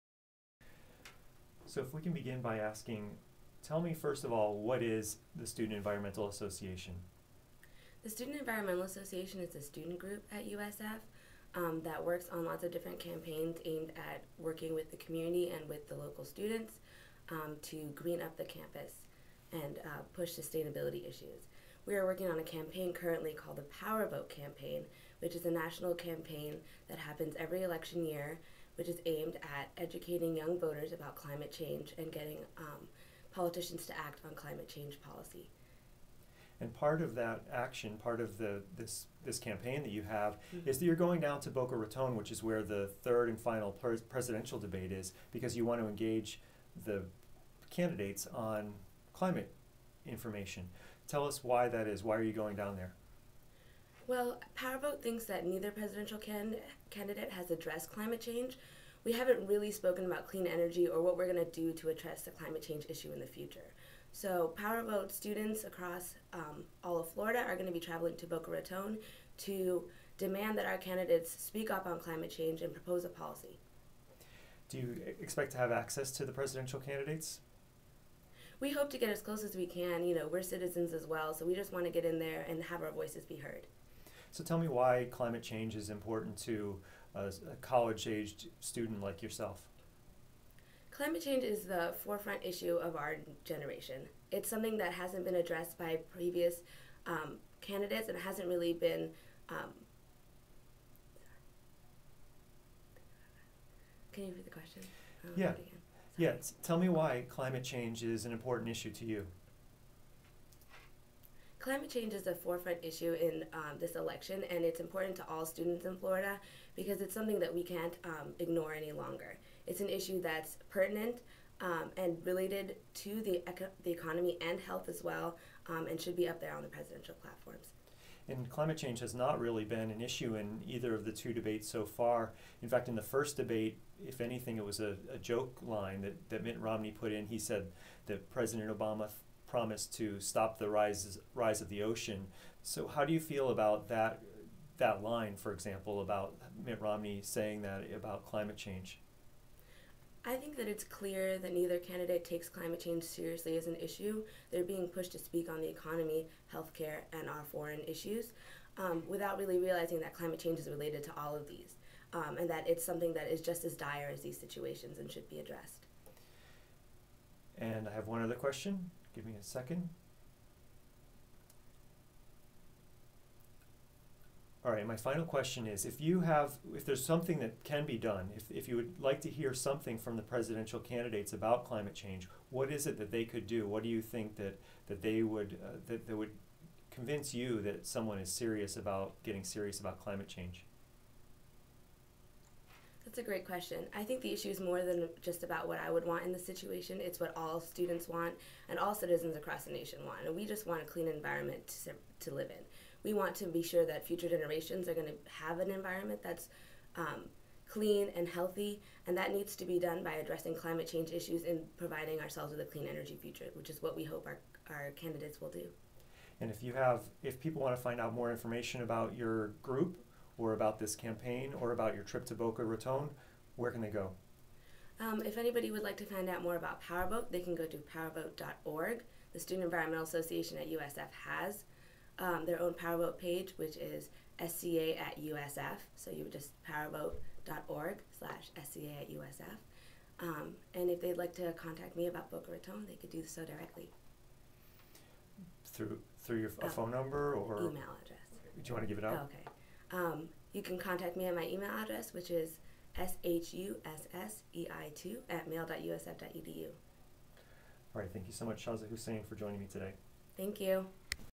USF students to press presidential candidates on climate change: WMNF News
Interview with Shaza Hussein from Student Environmental Association at USF